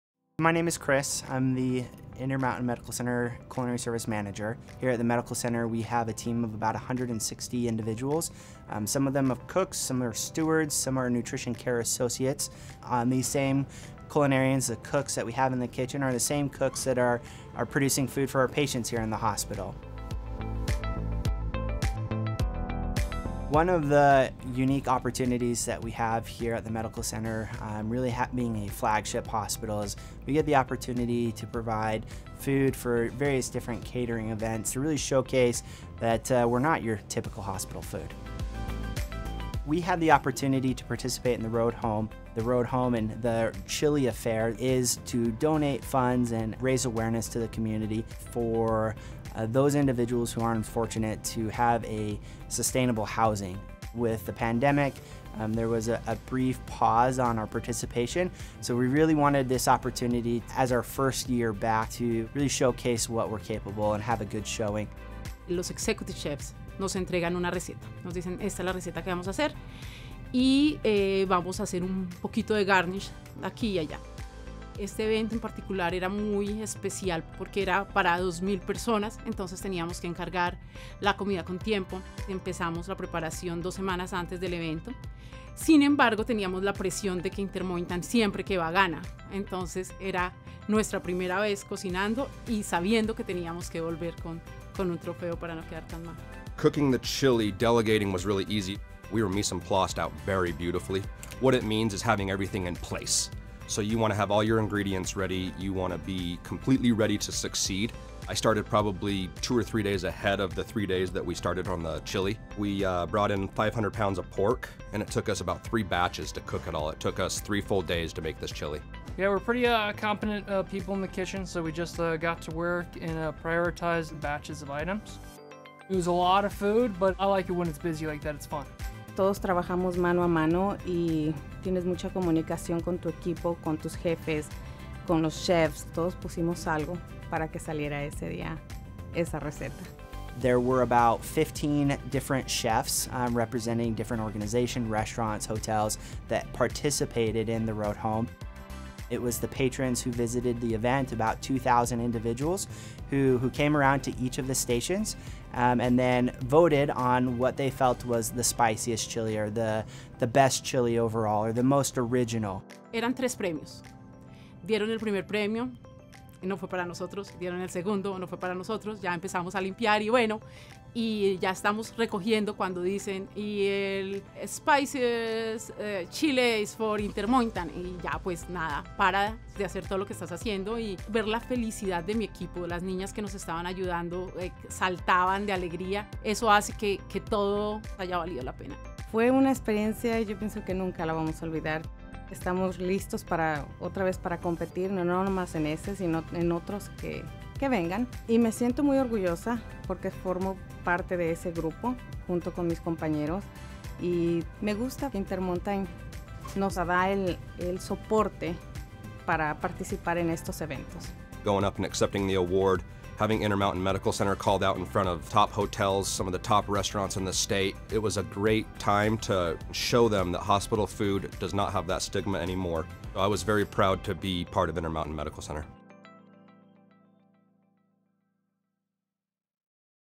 Intermountain Café proves it’s “Not Your Typical Hospital Food”
The culinary service team from Intermountain Café at Intermountain Medical Center received the Spiciest Chili Award from the 36th Annual Great Salt Lake Chili Affair. The event raises funds and awareness for The Road Home, an organization which helps individuals step out of homelessness. The team, including two chefs de cuisine, prepared food for 2000 patrons, which took three full days and included cooking 500 pounds of pork.

“To have Intermountain Medical Center called out in front of some of the top hotels and top restaurants in the state — it was a great time to show them that hospital food doesn’t have a stigma anymore,” says Tyler Kay, Chef de Cuisine I